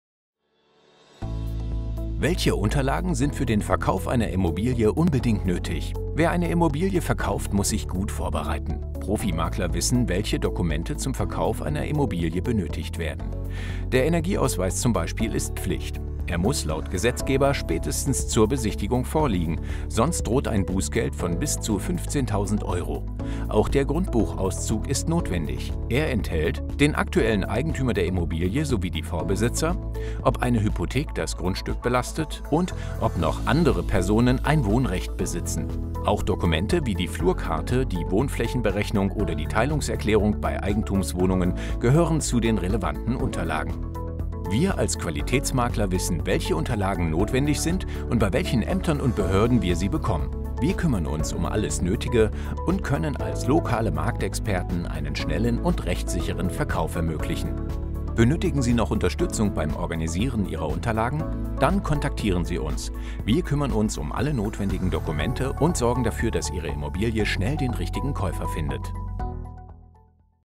Welche Unterlagen sind für den Verkauf einer Immobilie unbedingt nötig?
In diesem Video erhalten Sie eine Übersicht, welche Unterlagen für den Verkauf einer Immobilie nötig sind.
Christoph Samitz Immobilien e.K. in Eschborn